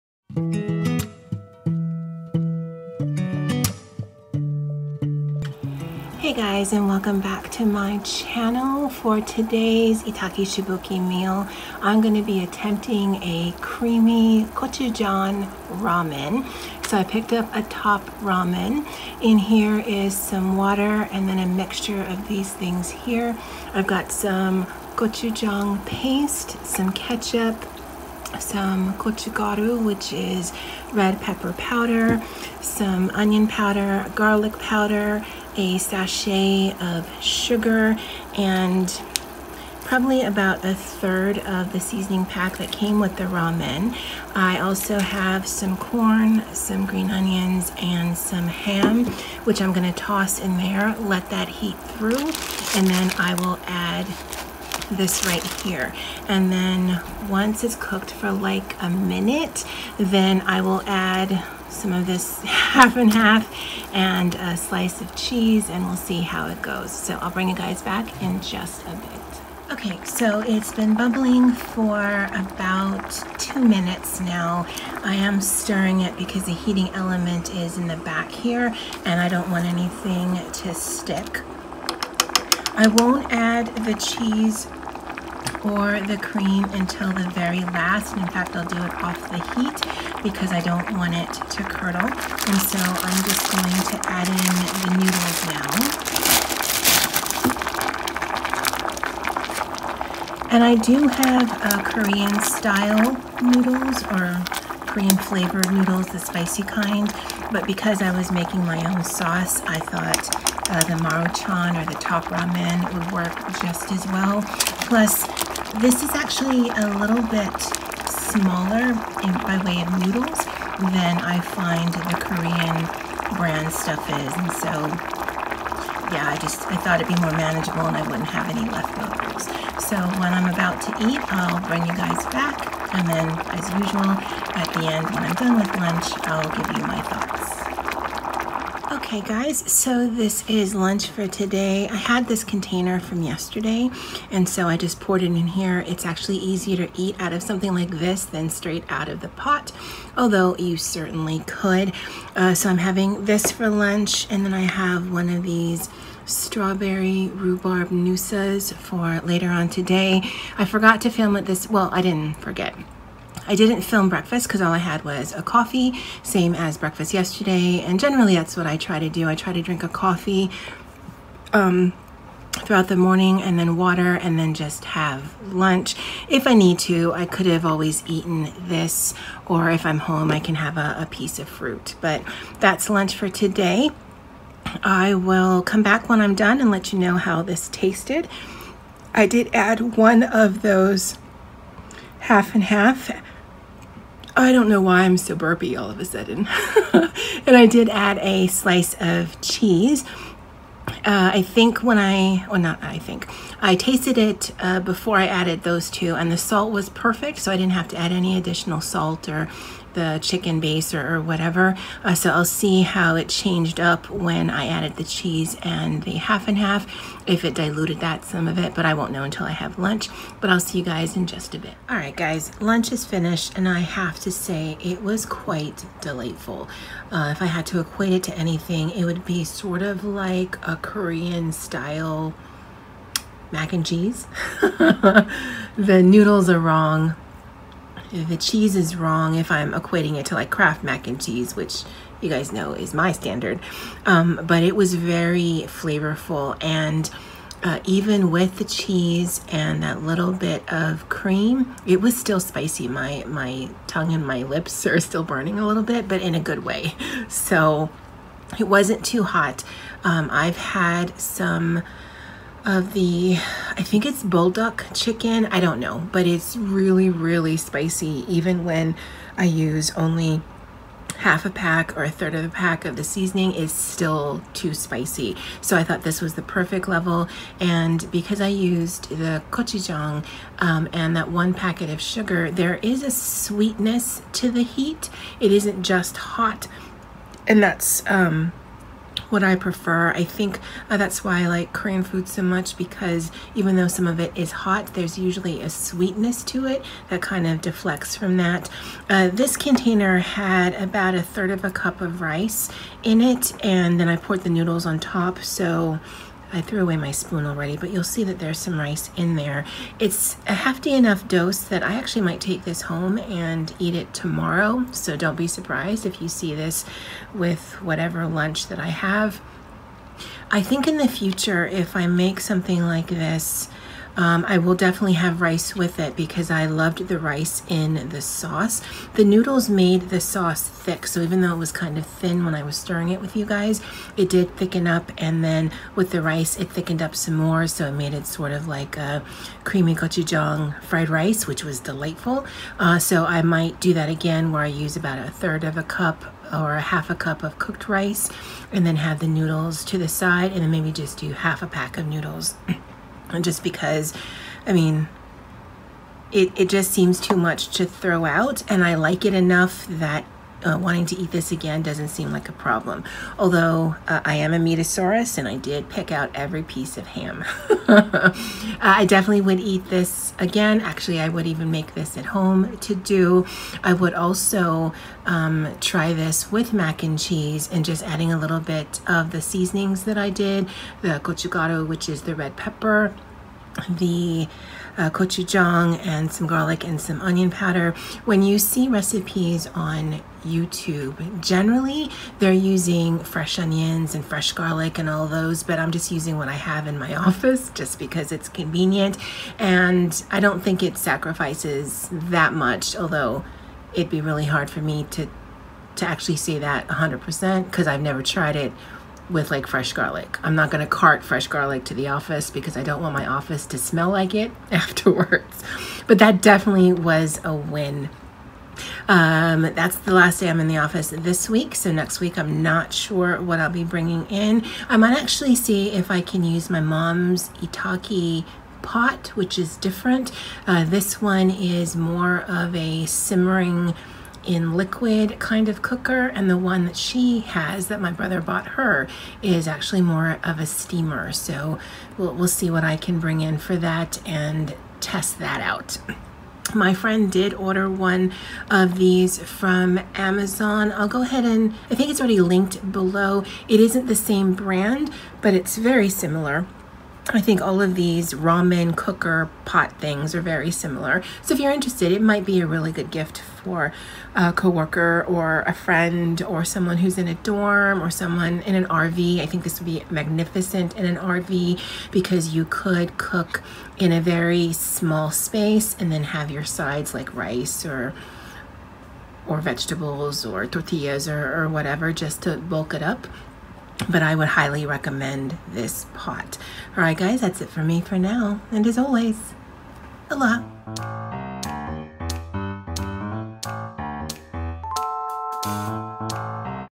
Easy Creamy Gochujang Ramen | Itaki Shabuki Pot | Cooking in Office
(late post, filmed Tuesday 10/20/20)

Give the gift of Prime
Try Audible and Get two free books

Similar to the pot shown